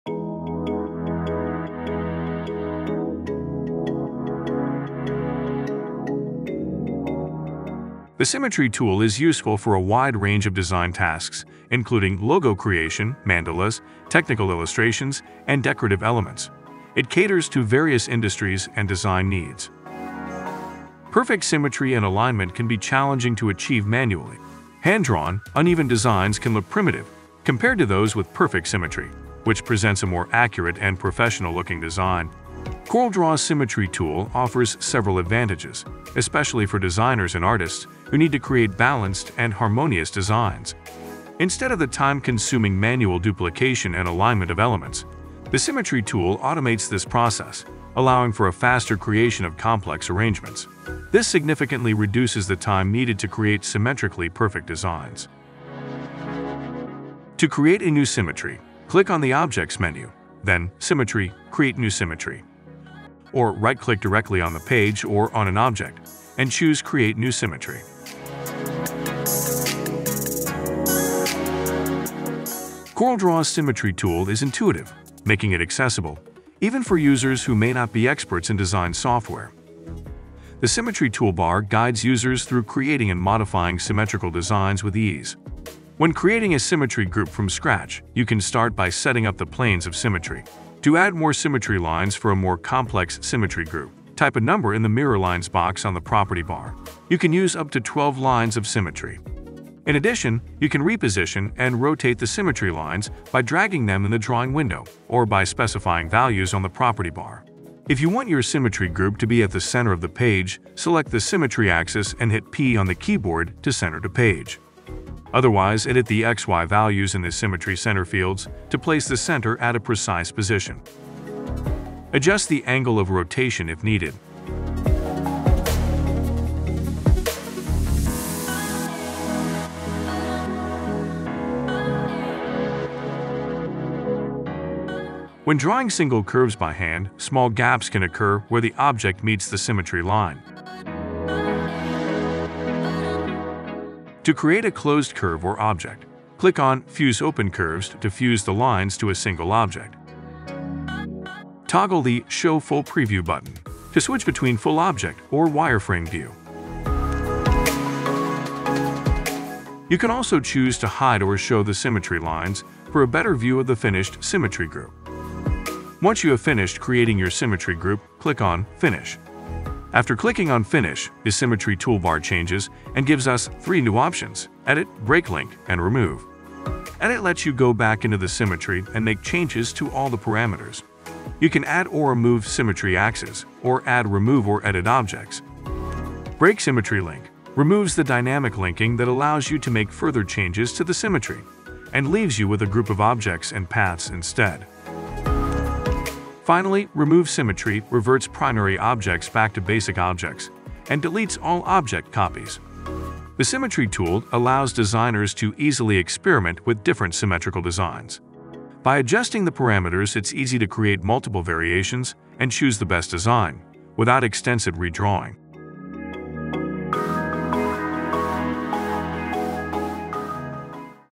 🎨✨ Tutorial: How to create perfect symmetrical designs in minutes with CorelDRAW's symmetry tool
From mandalas and logos to icons and decorative patterns, symmetry plays a key role in clean, professional design. But trying to maintain perfect balance manually often turns a simple concept into a complicated process.

This tutorial breaks down how the Symmetry Tool in CorelDRAW simplifies symmetrical drawing, allowing designers to focus on creativity while the software handles the duplication and alignment in real-time.

With just a few clicks, shapes, strokes, and details mirror across axes automatically. You can build entire symmetrical compositions, like mandalas, mirrored illustrations, or abstract layouts, faster and more precisely than ever before.

Key Features of the Symmetry Tool:

1. Perfect Symmetry Made Easy: Achieve balanced, harmonious designs without the hassle of manual alignment. Ideal for creating logos, mandalas, technical drawings, and more! 🏗️✨
2. Faster Design Creation: Say goodbye to time-consuming manual duplication. The Symmetry Tool automates symmetry creation, drastically reducing the time spent on complex arrangements. 🕒
3. Customizable Symmetry Lines: Create and modify symmetry groups with up to 12 symmetry lines. Adjust, reposition, and rotate lines to fine-tune your design. 🔄
4. Simple, Intuitive Interface: The Symmetry Tool is user-friendly, making it accessible to both beginners and seasoned professionals. With the toolbar guiding you, it's easy to create symmetrical designs in no time. 🛠️
5. Advanced Control: Control symmetry center placement, adjust the angle of rotation, and use the Fuse Open Curves feature to create closed objects with ease. 🖌️
6. Preview & Edit Options: Toggle between full-object and wireframe views and make on-the-fly edits to your symmetry, such as adding/removing axes or objects. 🔍
7. Dynamic Link Control: Use options like Break Symmetry Link to remove dynamic linking or Remove Symmetry to revert objects back to their basic form. 🔗

Why use the symmetry tool in CorelDRAW?

- Increase Productivity: Create symmetrical designs without redrawing elements, allowing for faster project completion.
- Perfect Precision: Achieve professional-quality symmetry that adds accuracy and polish to your designs.
- Endless Design Possibilities: Adjust symmetry parameters and instantly view the results.

Whether you're a professional designer or just starting, the CorelDRAW Symmetry Tool makes creating symmetrical designs a breeze. Speed up your design process and ensure your work looks clean, precise, and polished every time.